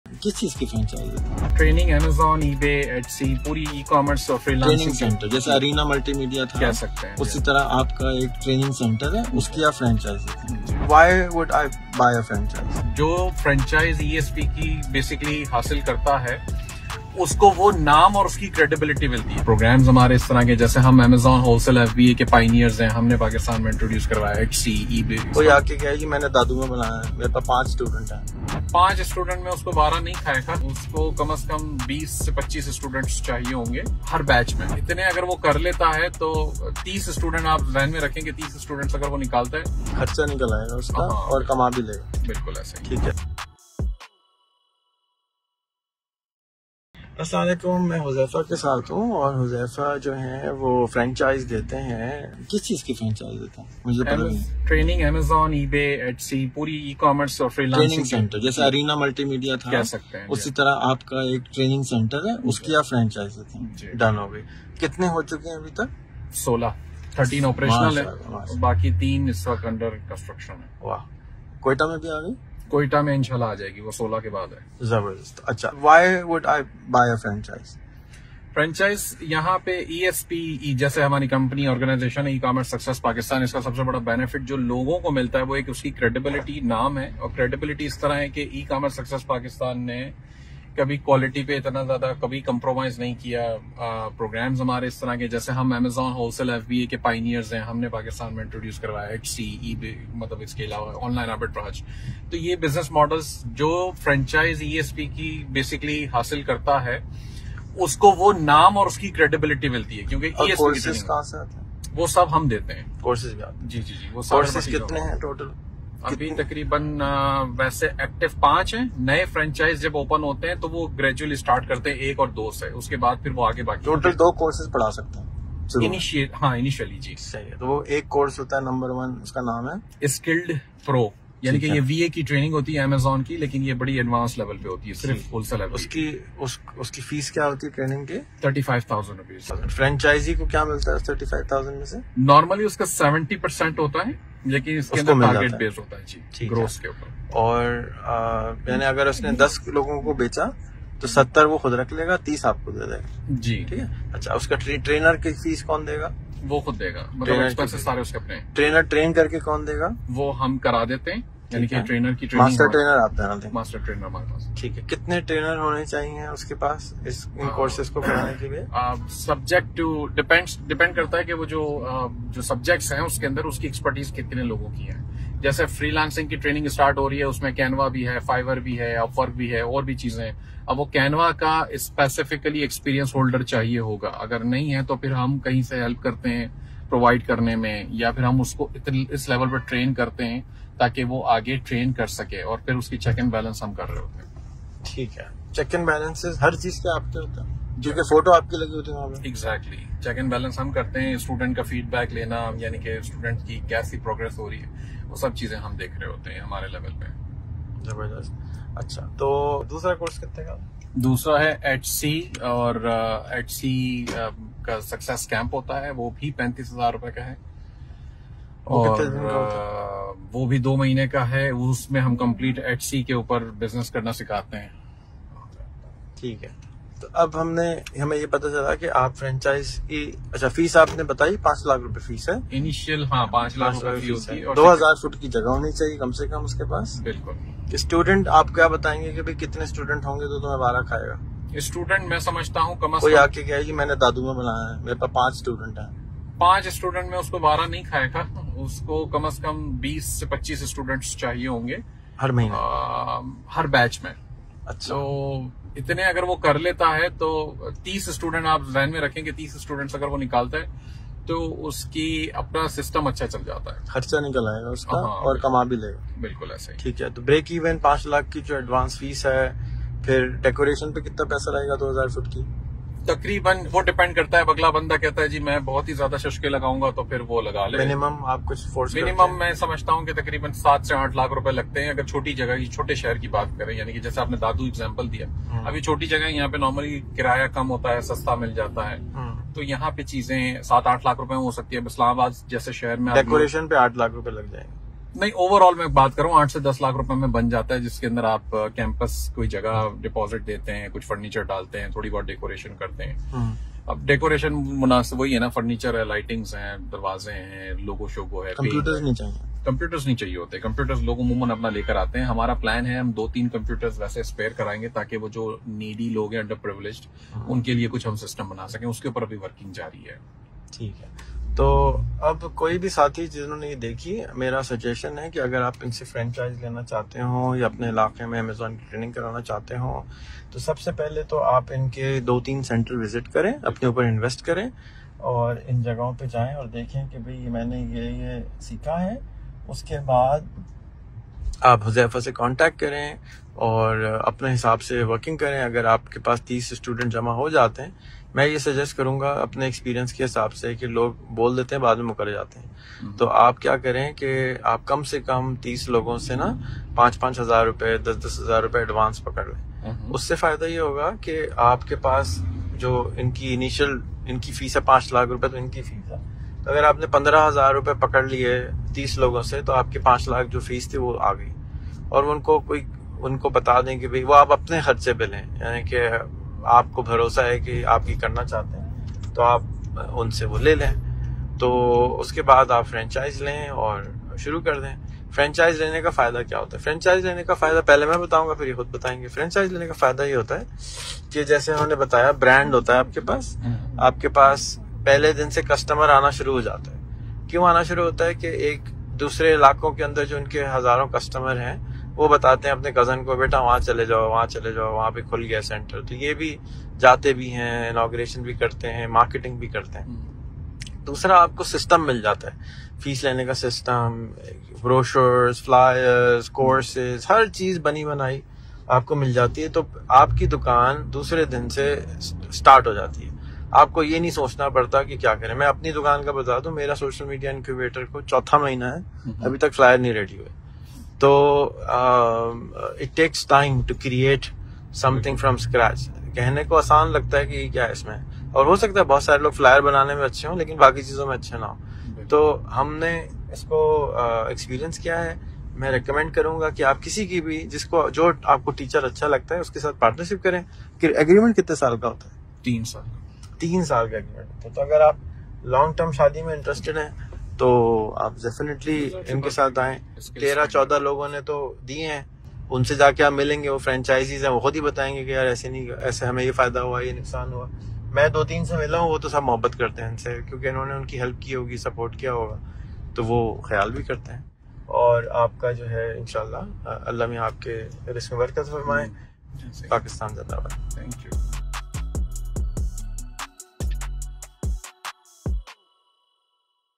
How To Open Franchise Of Ecommerce Success Pakistan By Huzaifa Ali
In this video Rehan Allahwala talking with Huzaifa Ali, CEO & Founder of Ecommerce Success Pakistan (ESP), asking questions about his organization Ecommerce Success Pakistan (ESP) like If someone buy their franchise what benefits they will get form Ecommerce Success Pakistan, How anyone can open Ecommerce Success Pakistan franchise in Pakistan under 15 lakh rupees.
Huzaifa Ali, CEO & Founder of Ecommerce Success Pakistan (ESP), is a Pakistani national with years of experience in eCommerce, Amazon Brand Selling and other International eCommerce Channels and Software Development. Born and raised in Pakistan, he decided to serve Pakistan by sharing his knowledge and helping people build their eCommerce businesses on international markets such as Amazon, Etsy & eBay.
 Ecommerce Success Pakistan offering their franchise in 5 lakh rupees. When you buy their franchise you are able to offer two courses initially out of five, first one is Skilled Pro Amazon Virtual Assistant(VA) course and second one is Etsy Success Camp. Skilled Pro Amazon Virtual Assistant(VA) and Etsy Success Camp both courses have same fees which is 35000 rupees each.